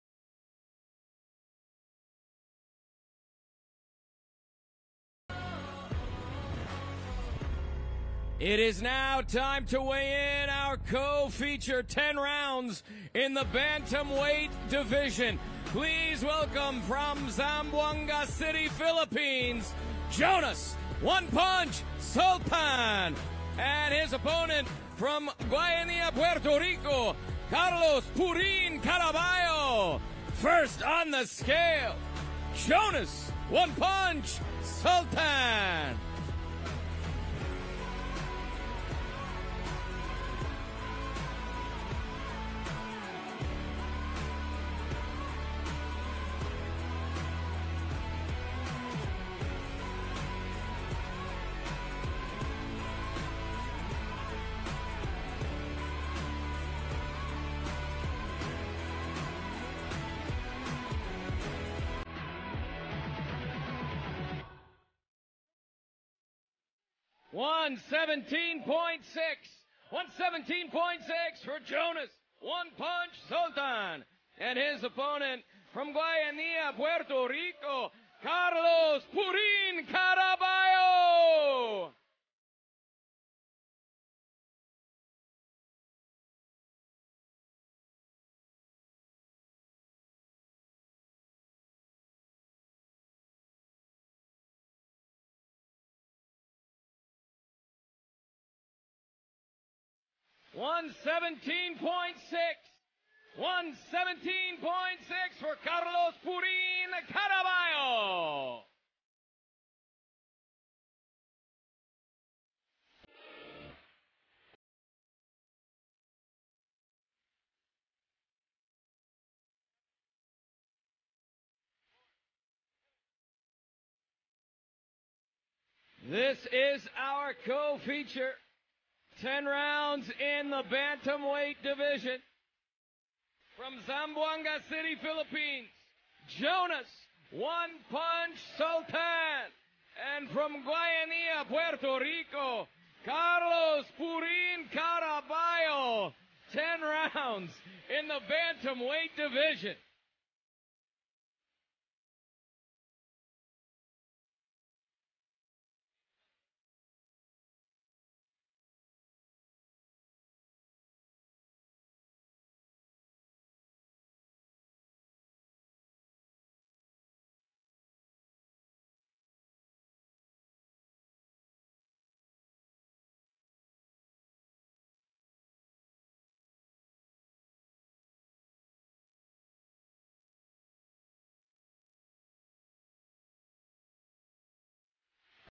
CARLOS CARABALLO VS JONAS SULTAN | FULL WEIGH IN & FACE OFF VIDEO
undefeated Carlos Caraballo takes on Jonas Sultan as the co main event of the Zepeda vs Vargas fight on ESPN Plus from NYC on Satuday October 30th! As we see Caraballo and Sultan both weigh in and go face to face!